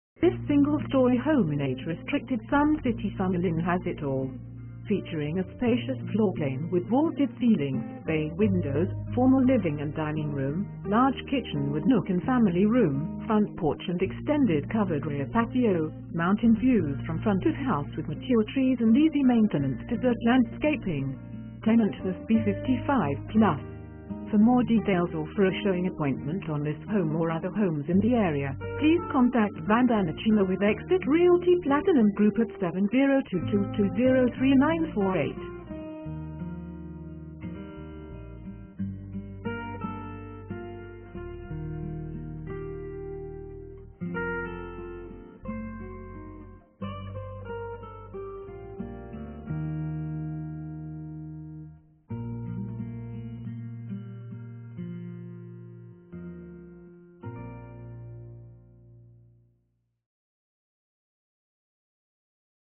A Single Story Rental in Age Restricted Sun City Summerlin
This single story home in Sun City Summerlin has it all. Featuring a spacious floorplan w/vaulted ceilings, bay windows, formal living & dining room, large kitchen w/nook & family rm, front porch & extended covered rear patio, mountain views w/mature trees & easy maintenance desert landscaping. Tenant to verify all info. Must be 55+.